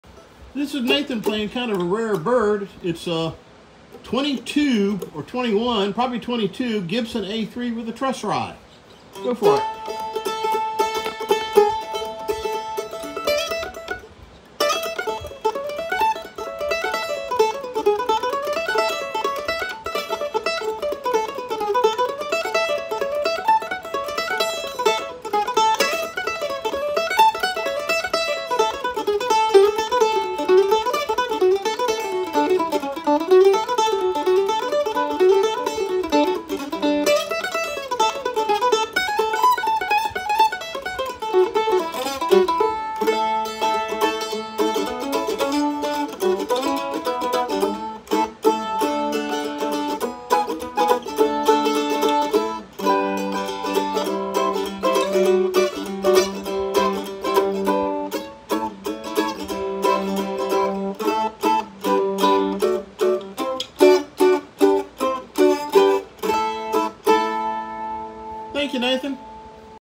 1922 Gibson A3 Mandolin
1922 Gibson A3 w/OHSC
Original truss rod instrument - very rare

Available from Mandolin World Headquarters, Inc.